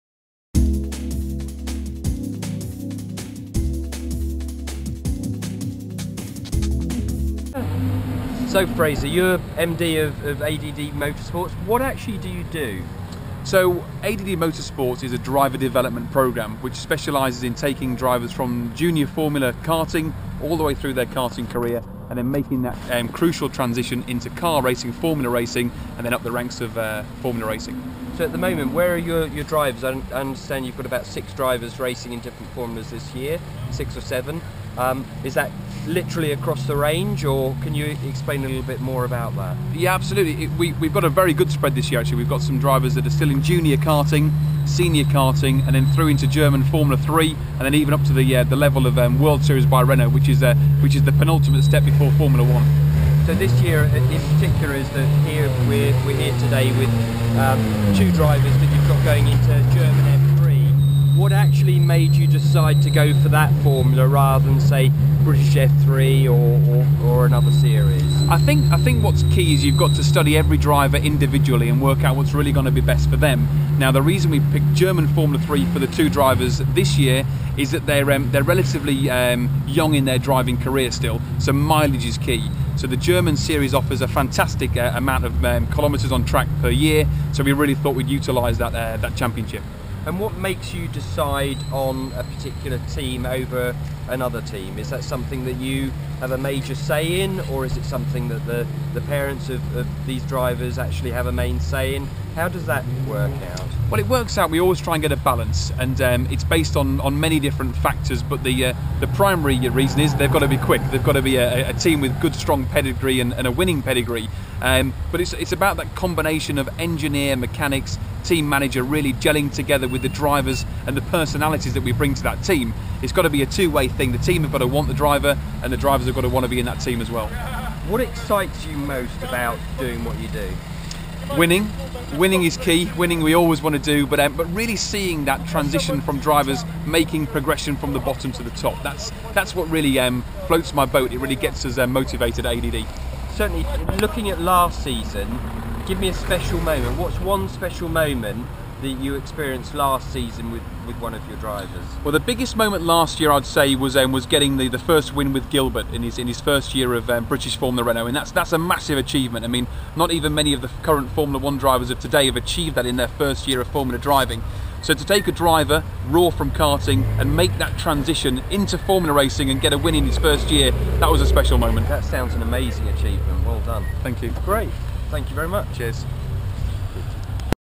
NBG | ADD Motorsports Interview.m4v
NBG Extreme interview with ADD Motorsports Director - Fraser Sheader